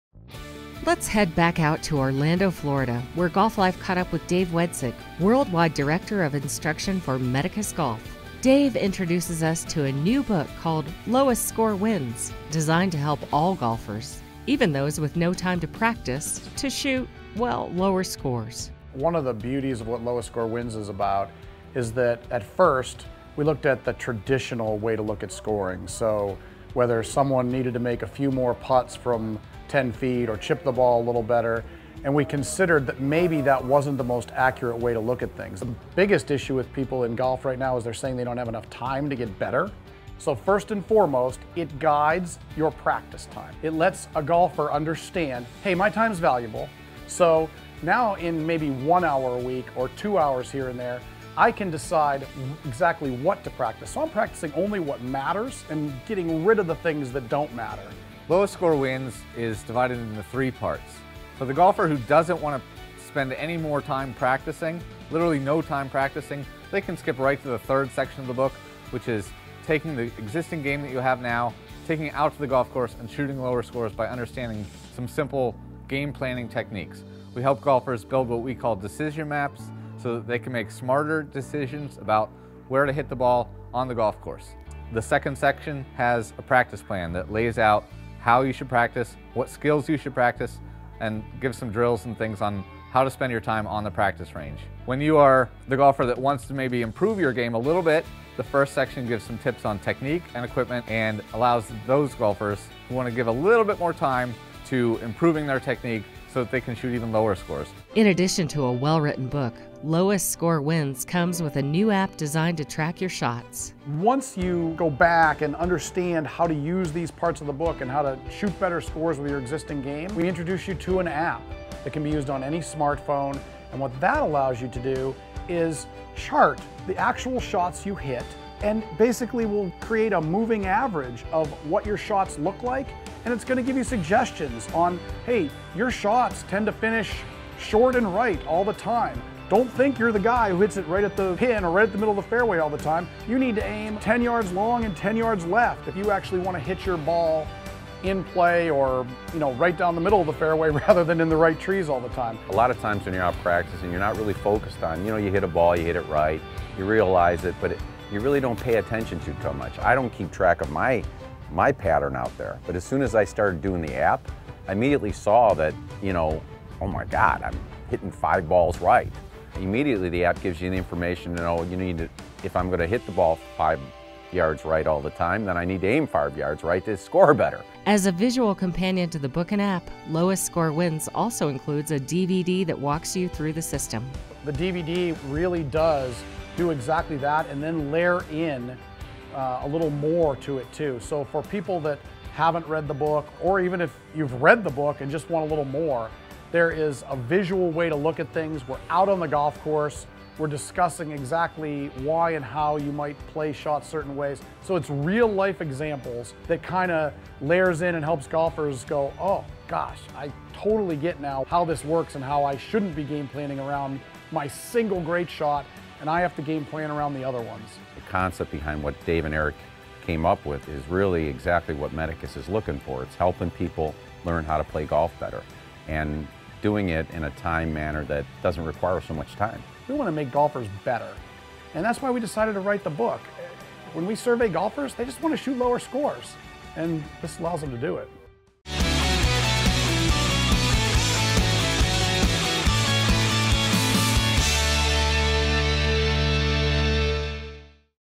Shoot Lower Scores with New Golf Book
Golf Life met up with Dave Wedzik and Erik Barzeski, co-authors of the book "Lowest Score Wins" at Reunion Resort to learn more about their new book.

Golf is unique: it’s the only sport in which the Lowest Score Wins. Golfers have been inundated for years with advice and sayings intended to help them shoot lower scores, like “spend 50% of your time practicing your putting” or “you’ve got to be in the short grass.”

What if we told you that most of this popular advice was not true at all? That these myths are holding you back from reaching your potential and shooting the lowest score possible? That putting might be the least important skill in golf, that driving the ball far is much more important than driving it straight, and that Phil Mickelson might just be the best strategist on the PGA Tour?

Lowest Score Wins is NOT your classic golf book. We show you the new way to shoot lower scores — immediately. You’ll learn to use something called Separation Value to guide your practice and how you can use Shot Zones to help you determine your GamePlan for every shot you play. You’ll discover why typical course management strategy fails (hint: it only covers half of the equation). This book is the first of its kind. It is your own personal roadmap to shooting lower scores tomorrow. What are you waiting for?